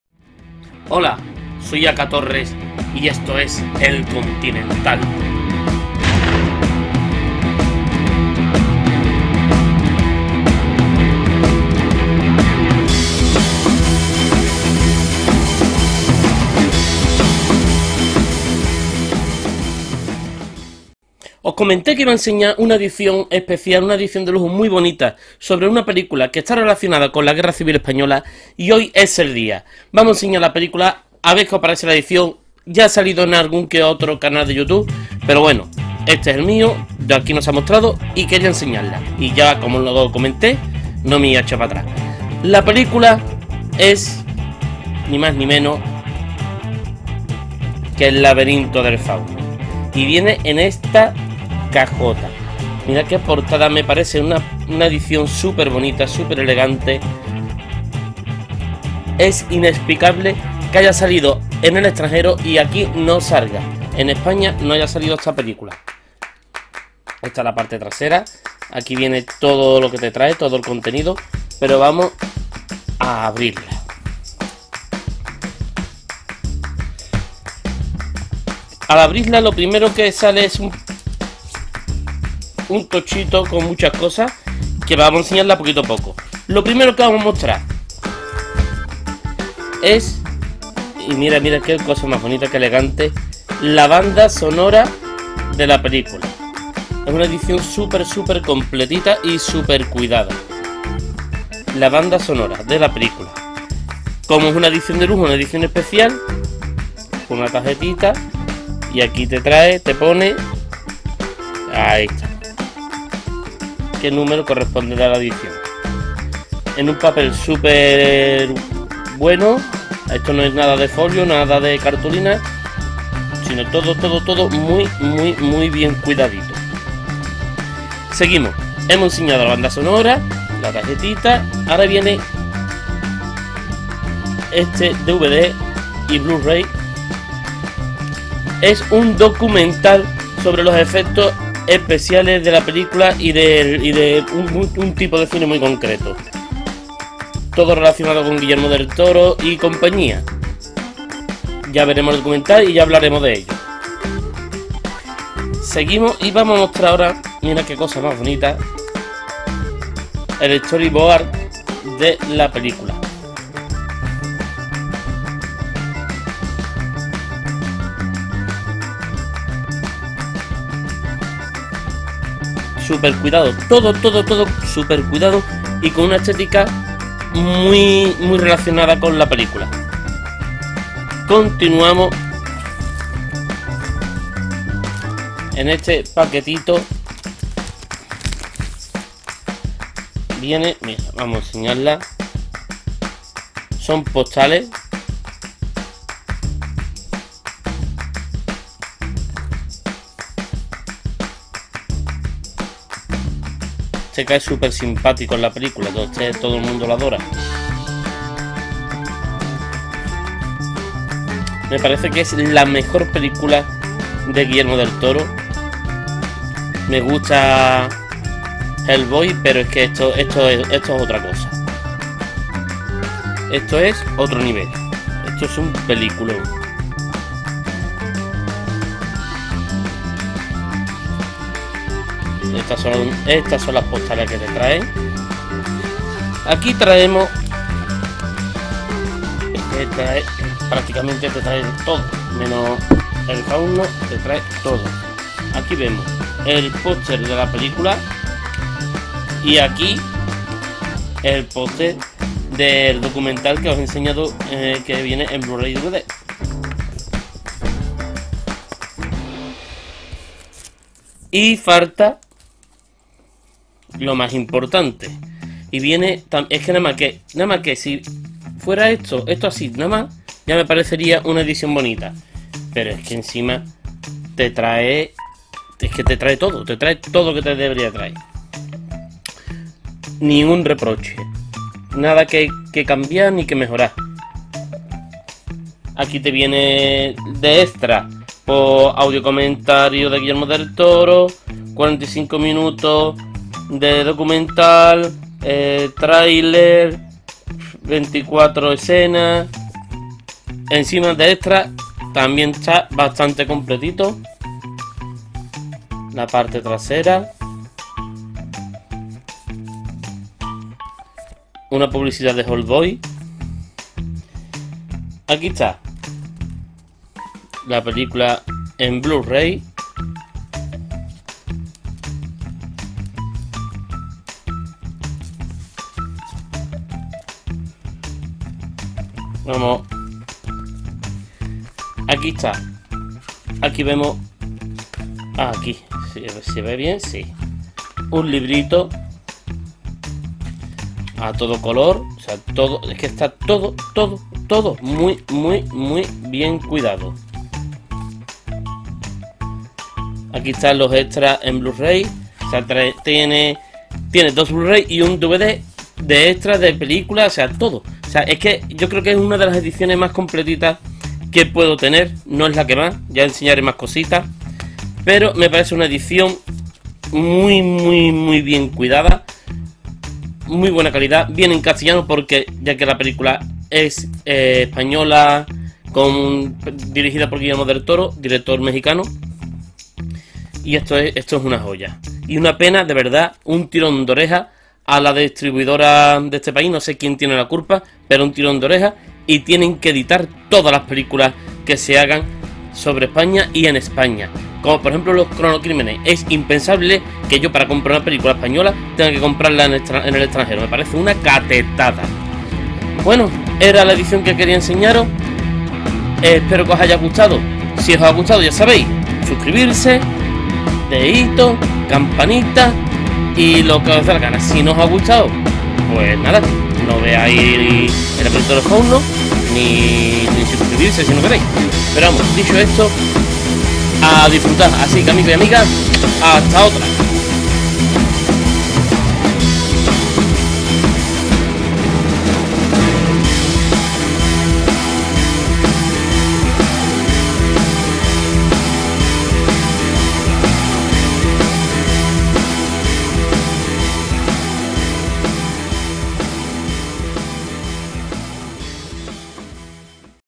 Coleccionismo.10. Blu-ray y 4k. Mis ultimas adquisiciones.Edicion de lujo. El laberinto del fauno .
Muestro mi colección en bluray y 4k...steelbook, digibook.